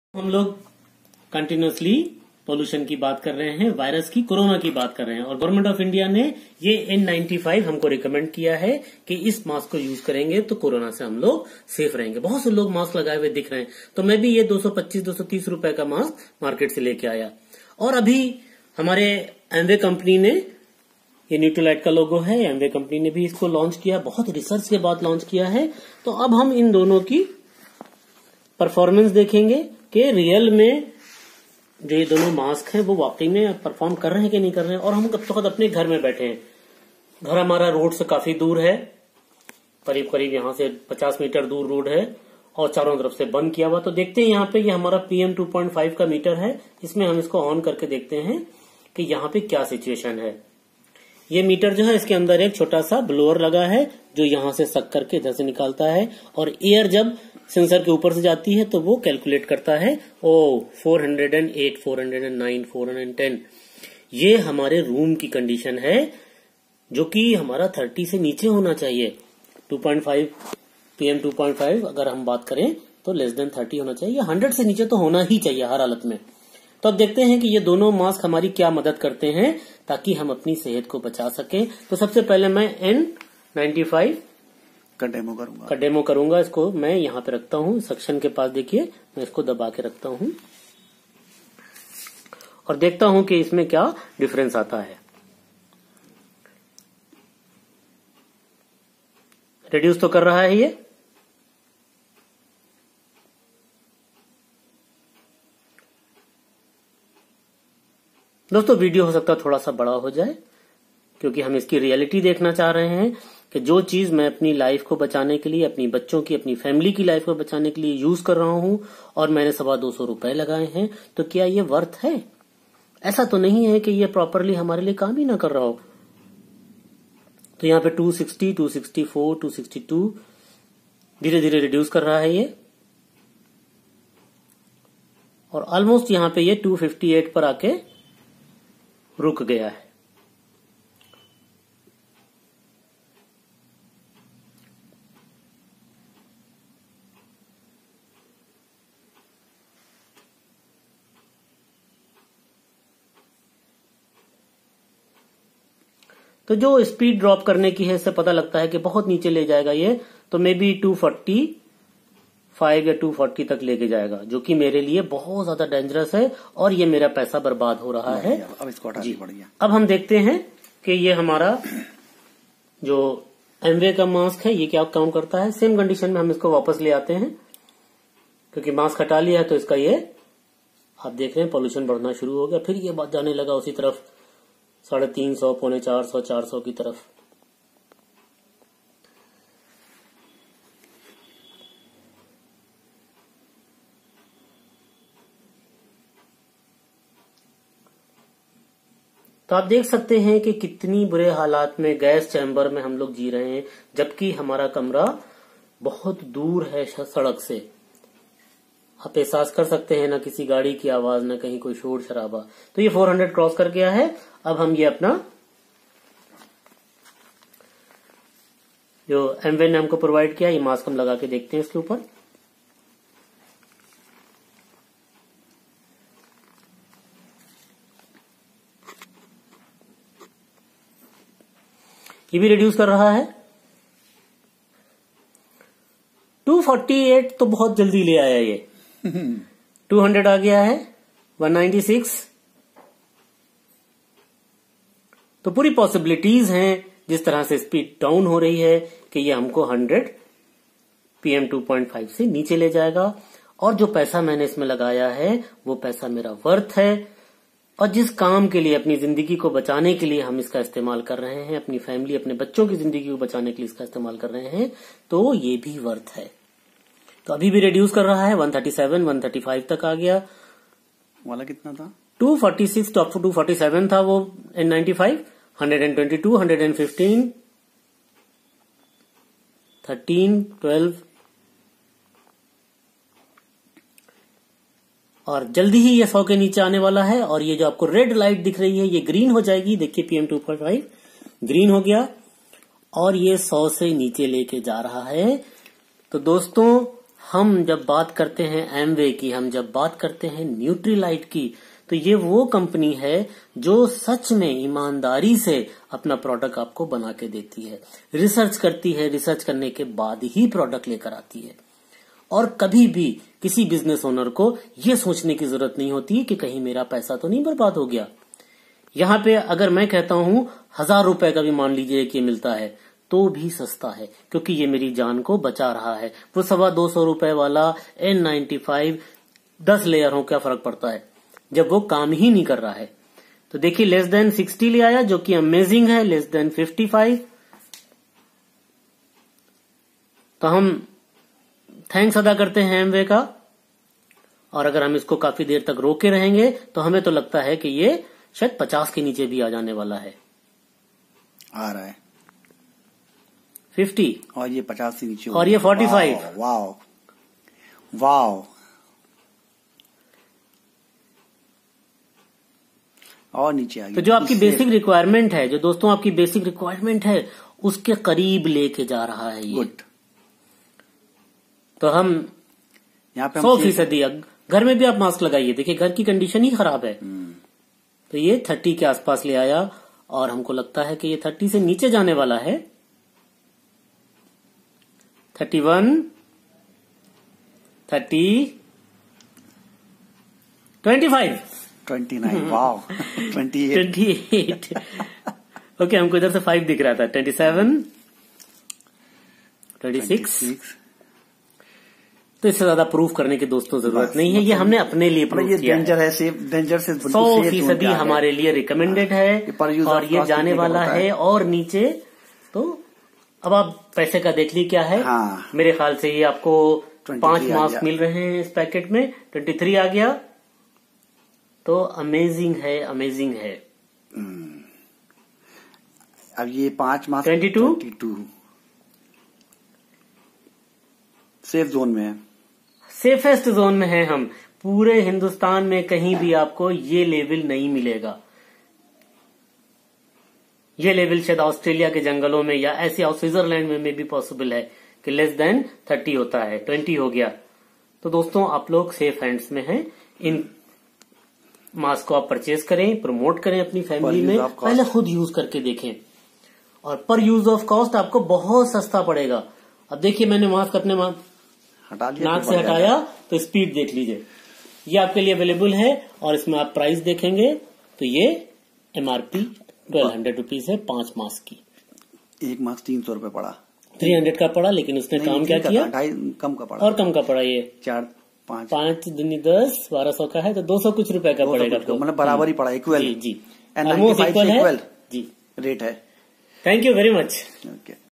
AMCAM- Face Mask Demo - Amway Nutrilite
Live Demo of Amway Nutrilite Face Mask - Why we are using this wonderful product to protect our beloved family.?